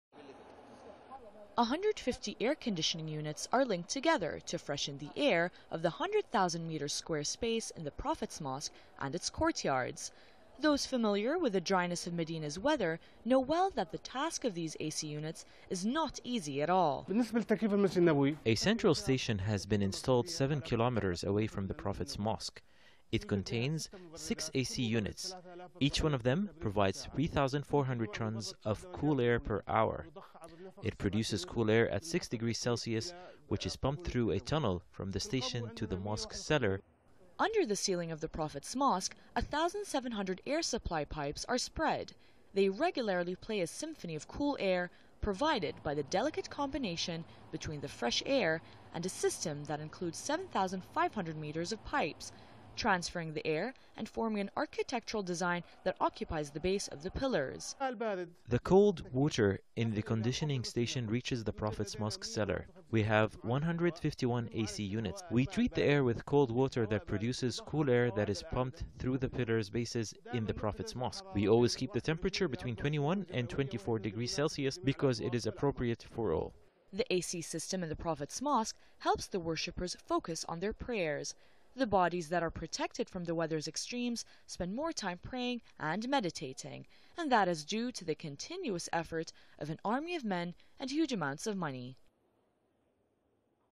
An enormous AC system providing cool fresh air to the Prophet's mosque
150 air conditioning units are linked together to freshen the air of the 100000m² space in the Prophet's Mosque and its courtyards. Those familiar the dryness of Medina's weather know well that the task of these AC units is not easy at all.

"As for the air conditioning process, a central station has been installed 7km away from the Prophet's Mosque. It contains six AC units; each one of them provides 3,400 tons of cool air per hour. It produces cool air at six degrees Celsius, which is pumped through a tunnel from the station to the mosque's cellar where we have 150 air processing units," senior project manager at the Prophet's Mosque, Fahad Hakim said.

Under the ceiling of the Prophet's Mosque, 1,700 air supply pipes are spread. They regularly play a symphony of cool air, provided by the delicate combination between the fresh air and a system that includes 7500m of pipes, transferring the air and forming an architectural design that occupies the base of the pillars.

"The cold water in the conditioning station reaches the Prophet's Mosque cellar. We have 151 AC units. We treat the air with cold water that produces cool air that is pumped through the pillars' bases in the prophet's mosque. We always keep the temperature between 21 and 24 degrees Celsius because it is appropriate for all," Hakim said.

The AC system in the Prophet's mosque helps the worshipers focus on their prayers. The bodies that are protected from the weather's extremes spend more time praying and meditating, and that is due to the continuous effort of an army of men and huge amounts of money.

By Khamis al-Zahrani
Al Arabiya
Medina